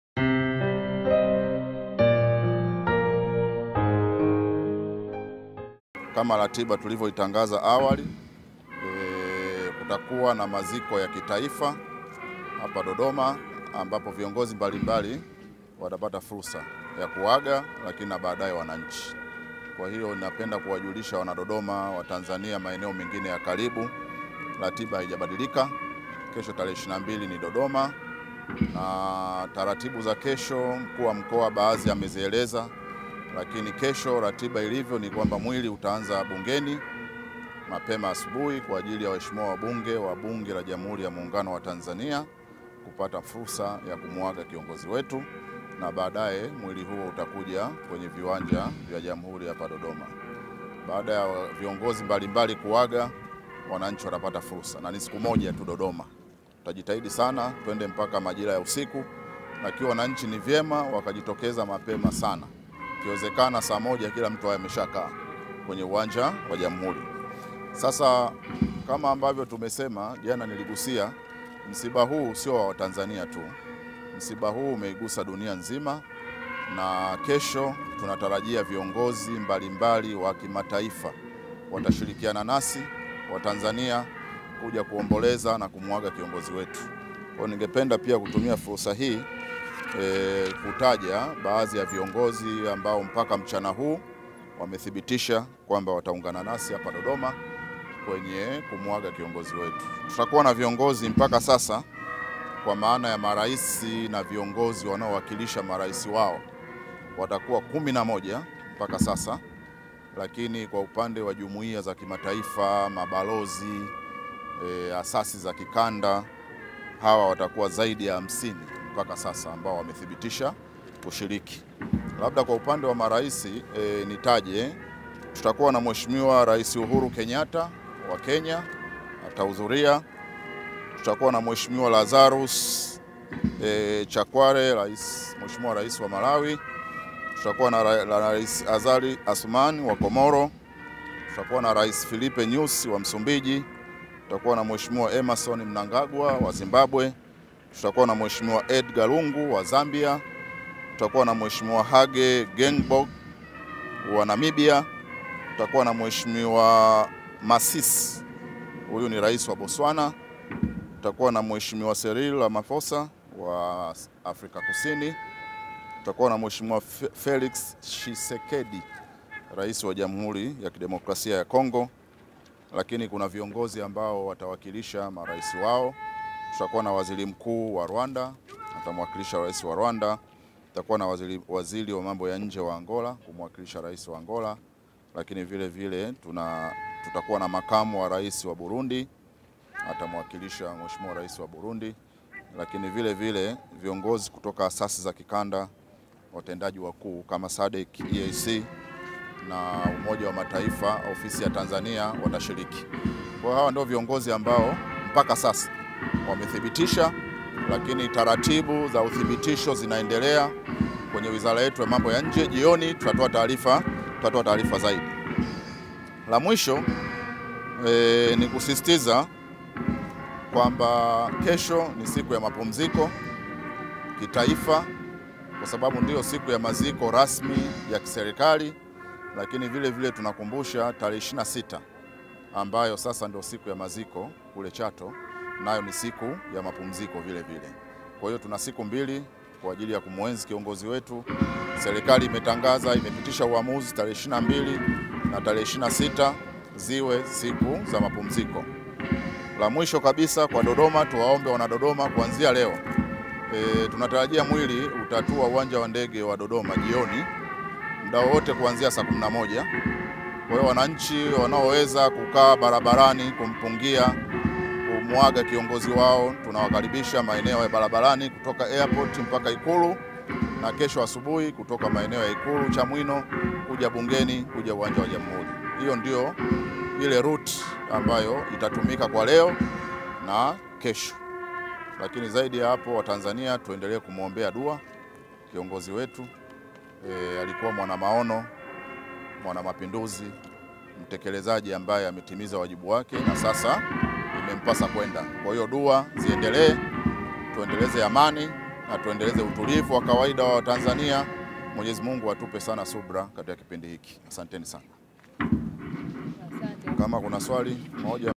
MWILI WA HAYATI DKT. MAGUFULI BUNGENI KESHO, VIONGOZI WA KIMATAIFA WATAKAOSHIRIKI KUAGA DODOMA
"Kesho tarehe 22 ni Dodoma, mwili utaanza bungeni mapema asubuhi kwa ajili waheshimiwa wabunge kupata fursa ya kumuaga kiongozi wetu baadae mwili huu utakuja kwenye viwanja vya Jamhuri hapa Dodoma, tunatarajia viongozi mbalimbali wa kimataifa watashirikiana nasi watanzania kuja kumuaga kiongozi wetu, baadhi ambao mpaka mchana wamethibitisha wataungana na sisi hapa Dodoma, tutakuwa na viongozi kwa maana ya marais na viongozi wanaowakilisha marais wao watakuwa 11 mpaka sasa lakini kwa upande jumuiya za kimataifa, mabalozi, asasi za kikanda hawa watakuwa zaidi ya 50 ambao mpaka sasa wamethibitisha kushiriki  "-Dkt.  Hassan Abbas